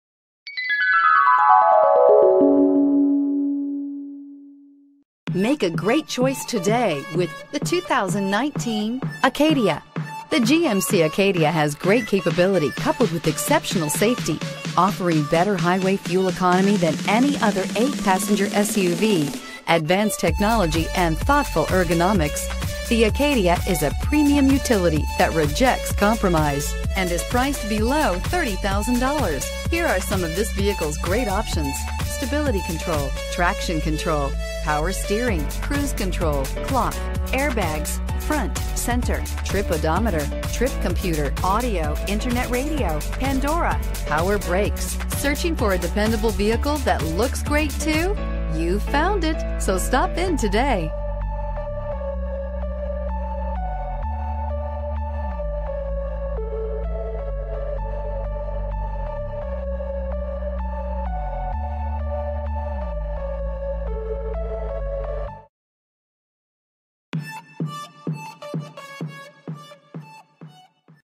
2019 GMC Acadia Huntersville NC GM9561
2019 GMC Acadia SLE-1

Pick up this Silver 2019 GMC Acadia, available today at Randy Marion Buick GMC. This could be the one you've been searching for.